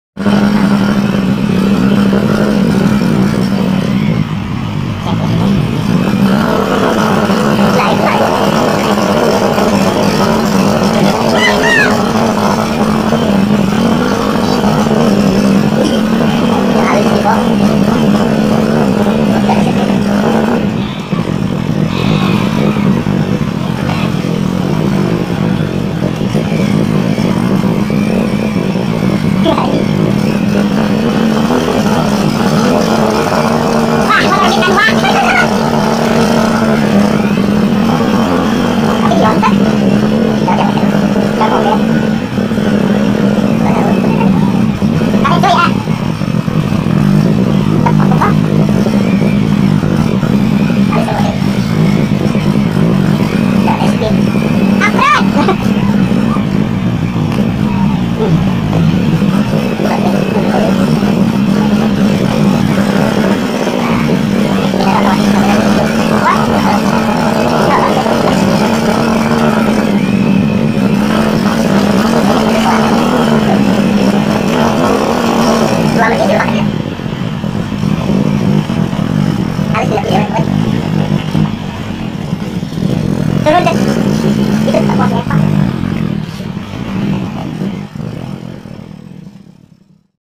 EPESODE OFFROAD # 5+5 EKSTRIM 2020 HAJAR !!!!! PART
NGETREK PALING MENEGANGA KAN JALAN YAH EKSTRIM BANGET